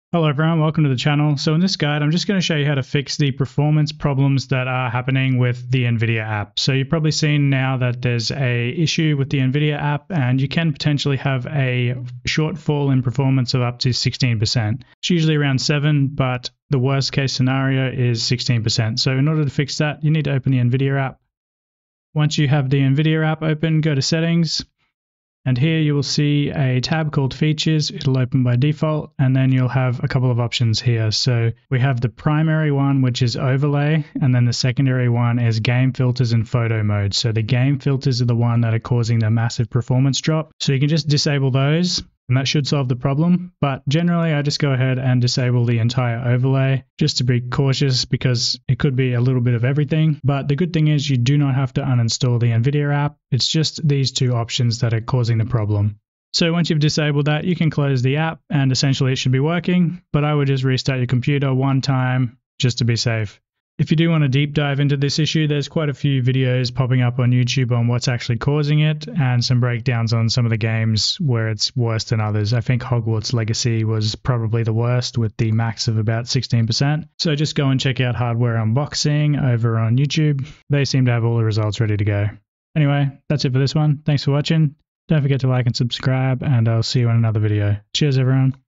How to Fix the NVIDIA App Gaming Performance Issues.
The Nvidia App, which is replacing both Nvidia Control Panel and GeForce Experience, is a nice new update but it seems to come with a small caveat: A performance hit when using certain overlay features. While not everyone is using filters if you are you may be taking a 16% performance hit doing so. So in this guide, we're going to guide you through the fixes until the issue is patched officially.